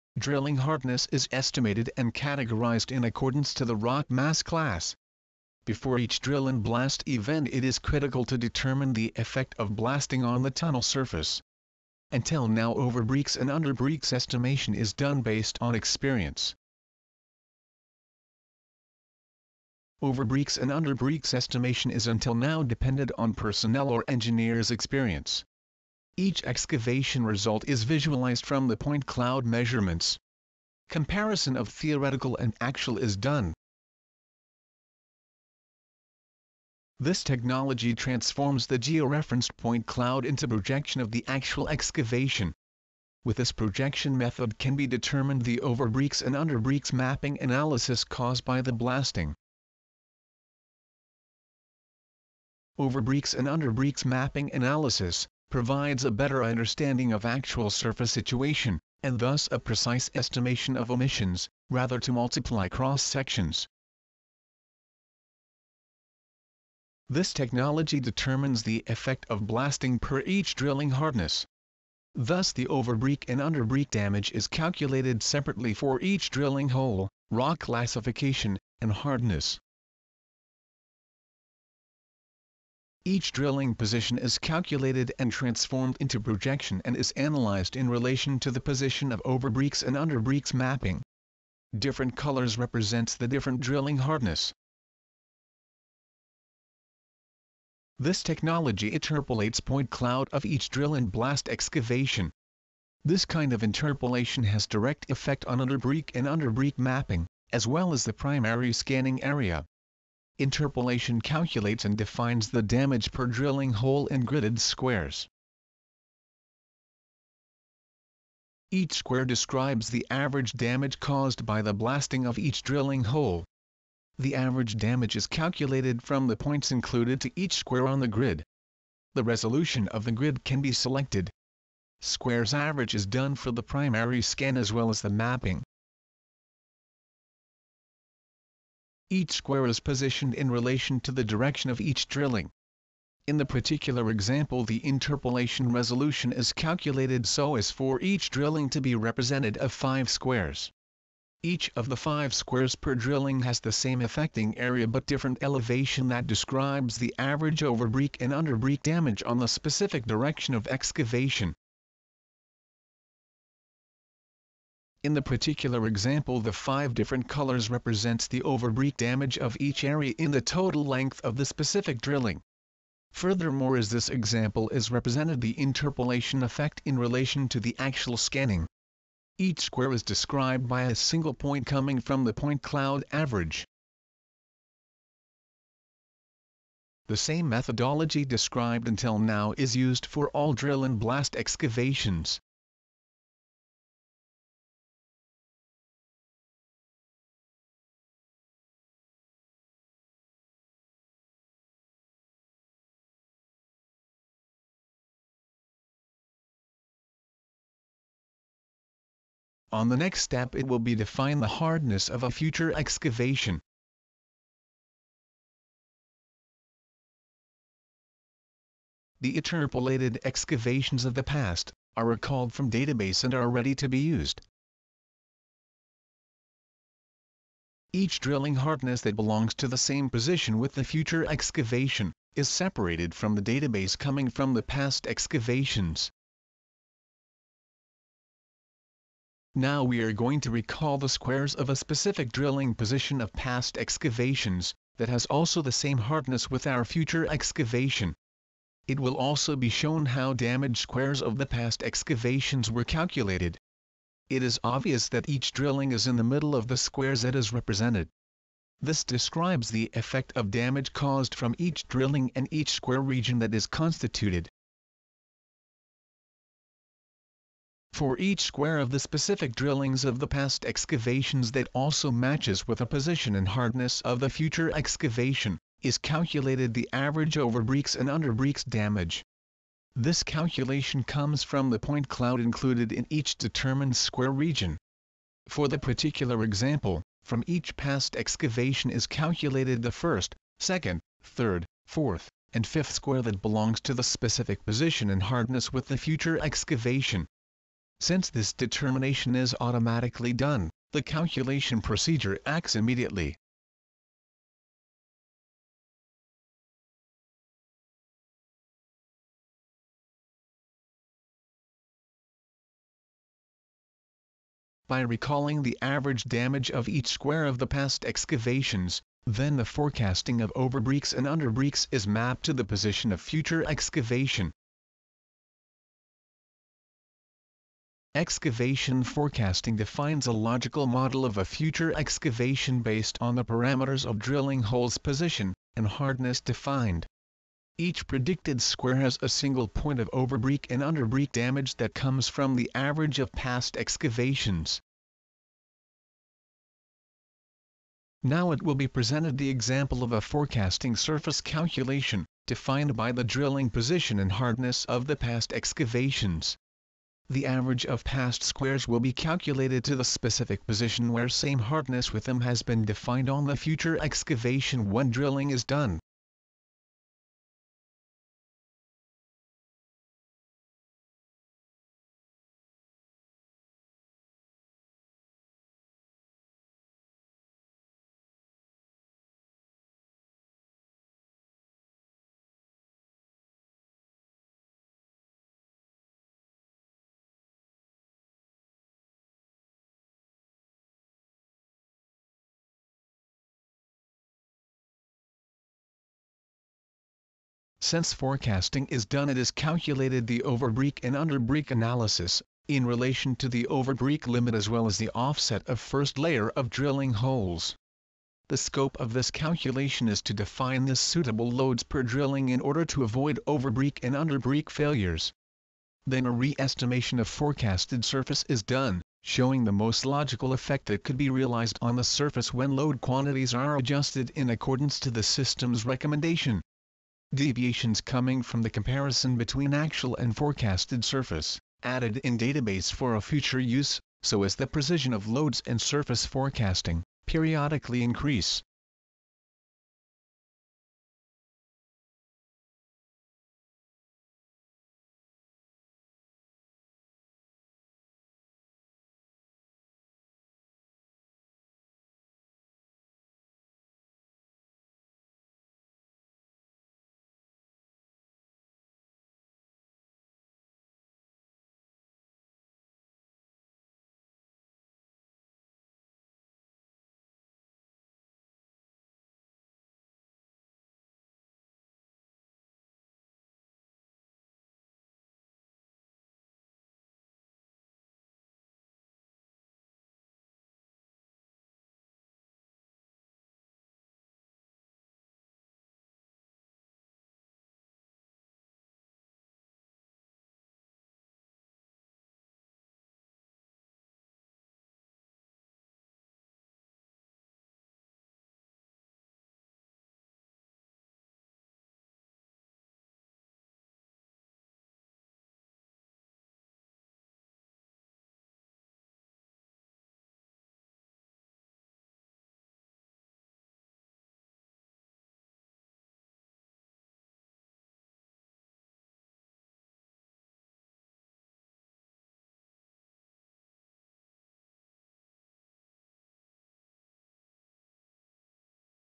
Excavation forecasting control during tunnel construction.
WHAT IS EXCAVATION FORECASTING FOR TUNNEL ENGINEERING?

Information for new products from geonovelty.
TBM segmental lining Survey

Next evolution on Surveying Tunnel Boring Machine.

Advanced best fit geometric comparisons and as-built analysis. Precision and quality like never before provided by Geonovelty.